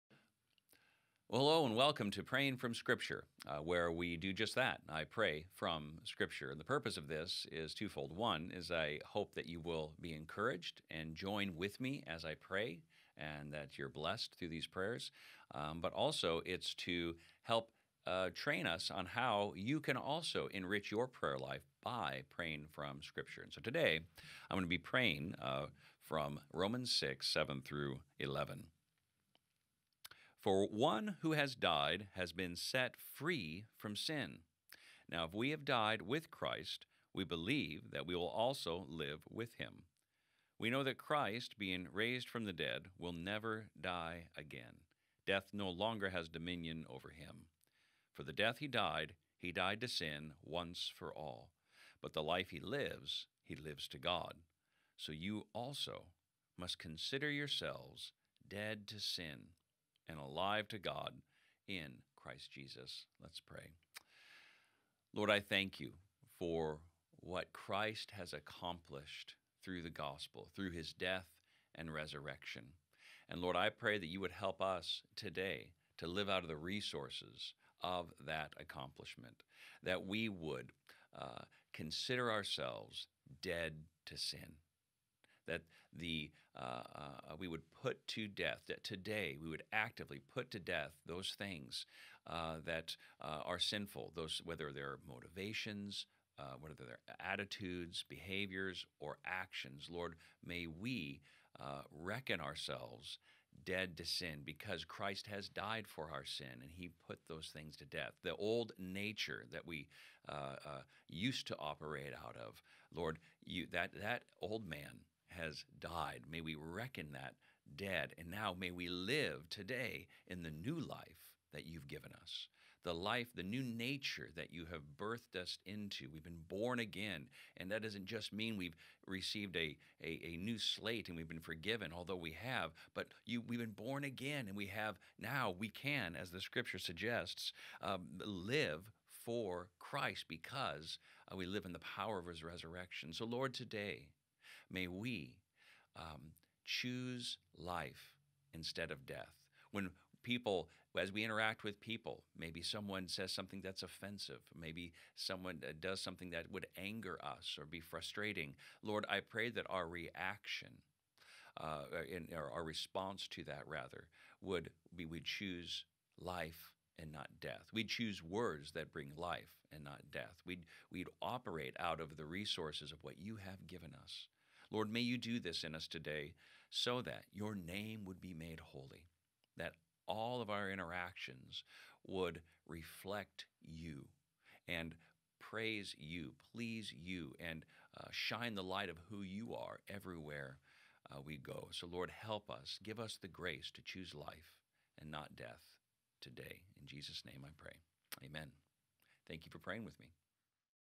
Romans 6:7-11 (ESV)
Join us today as we pray the words of Romans 6:7-11.

"For one who has died has been set free from sin. Now if we have died with Christ, we believe that we will also live with him. We know that Christ, being raised from the dead, will never die again; death no longer has dominion over him. For the death he died he died to sin, once for all, but the life he lives he lives to God. So you also must consider yourselves dead to sin and alive to God in Christ Jesus."